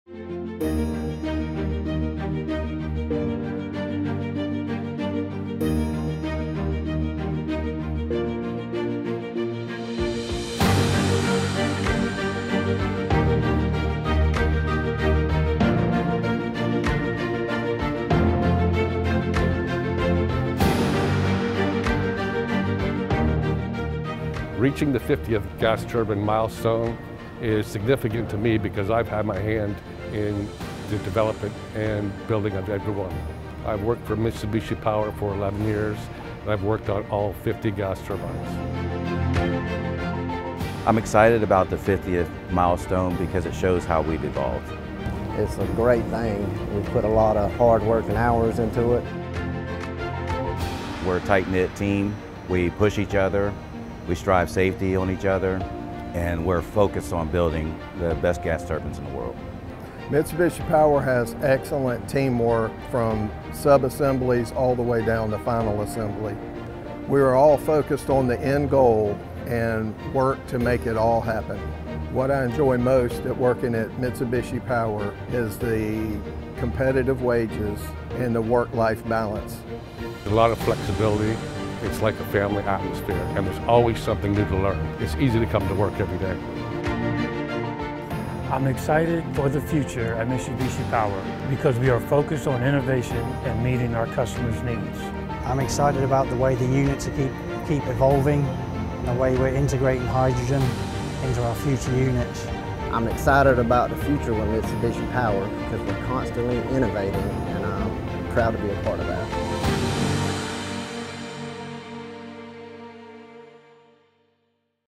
Celebrating 50 Advanced-Class Gas Turbines Shipped from Savannah Machinery Works
in June 2023 we celebrated the manufacturing and shipment of our 50th Advanced-Class Gas Turbine from our Savannah Machinery Works facility. This fleet is able to power over 3 million homes across 5 countries. Thank you to everyone who came out to help us celebrate this amazing milestone!